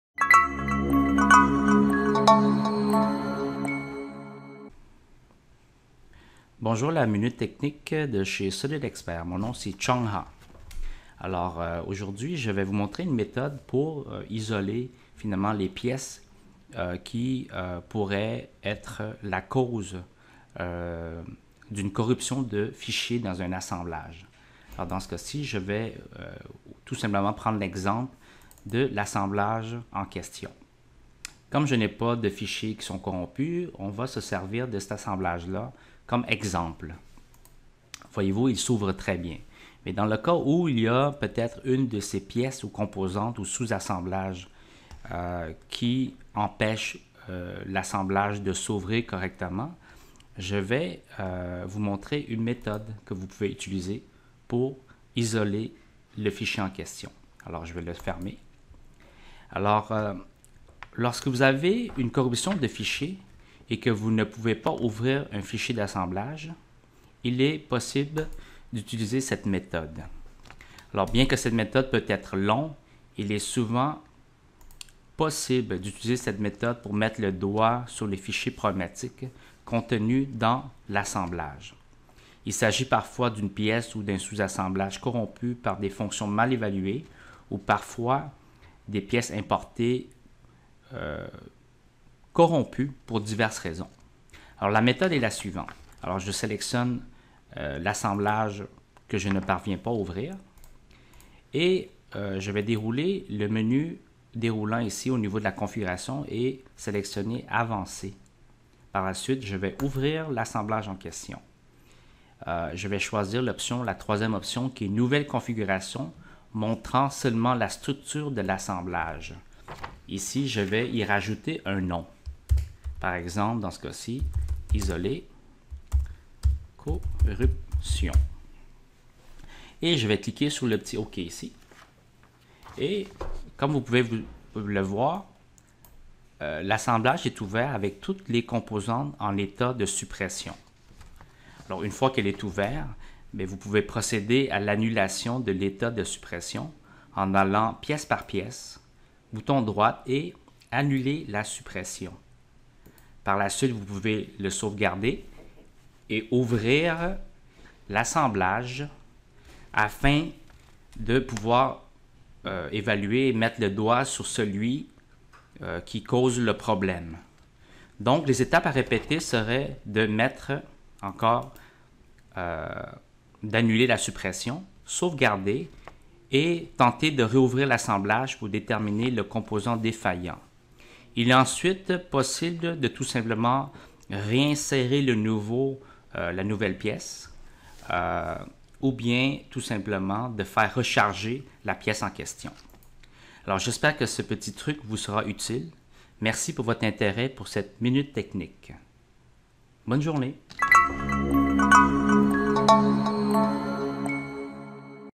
SOLIDWORKS 2018 - Isolation des composants corrompu - Mars 2018
Lorsque vous avez une corruption de fichier et que vous ne pouvez pas ouvrir un fichier d’assemblage. Il est possible d’utiliser une technique pour isoler la (les) composantes qui est peut-être en cause.